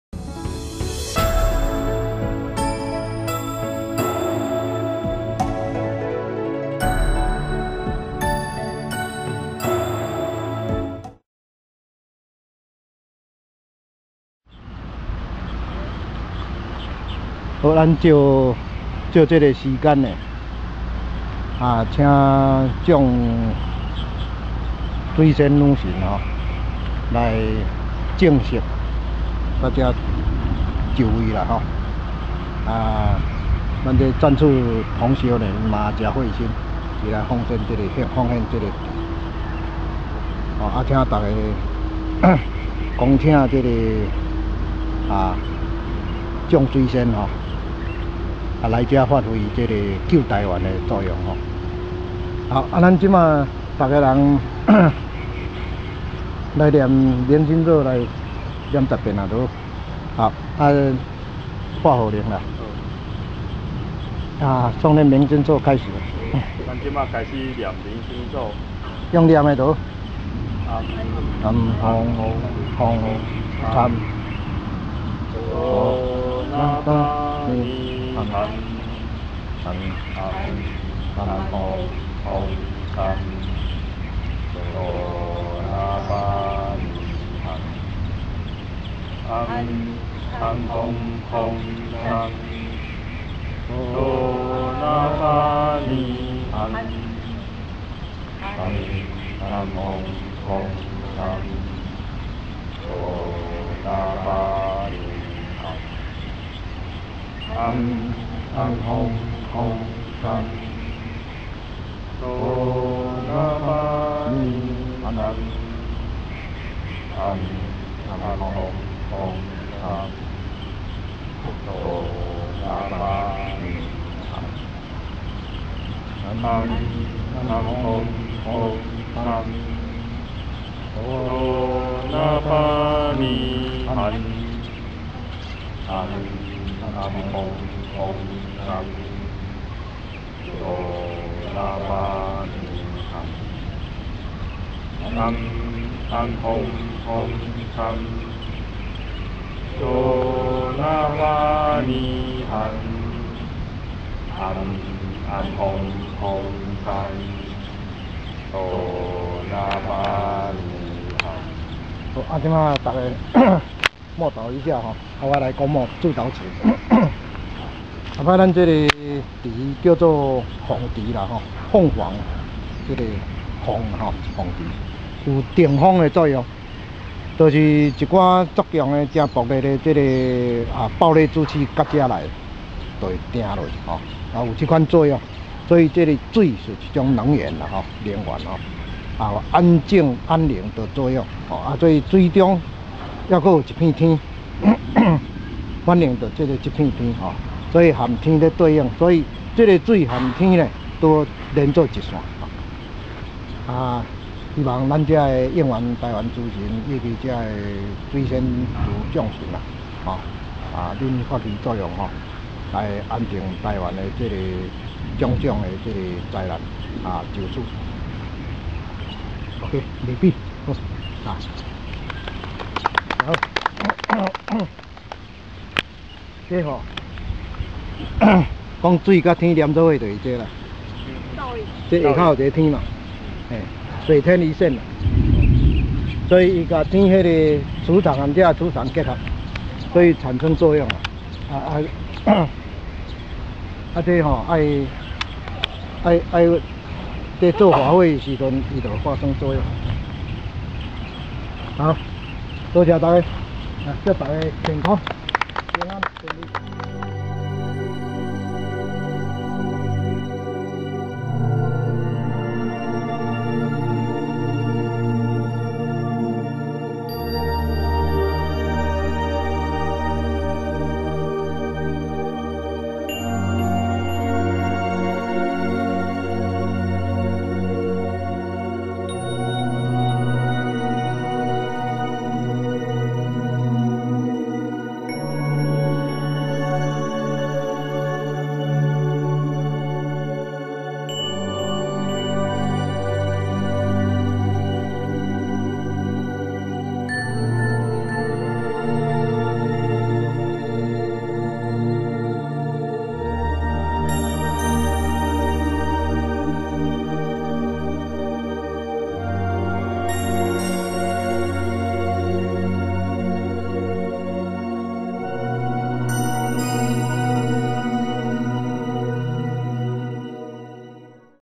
聖山－鳳池開光
2010年9月11日，恭請水仙正式坐鎮「鳳池」，發揮救台灣的作用，聖山的生態池取名為「鳳池」的原因，是希望有定風的作用，讓台灣的暴戾之氣止於鳳池，感謝贊叱同修的發心奉獻。

水是一種能源，有安靜安寧的作用，水中也有一片天，水天對映，連成一線，希望應元台灣神與眾水仙，能安定台灣的各種災難，水天磁場產生作用，聖山法會時將發揮作用。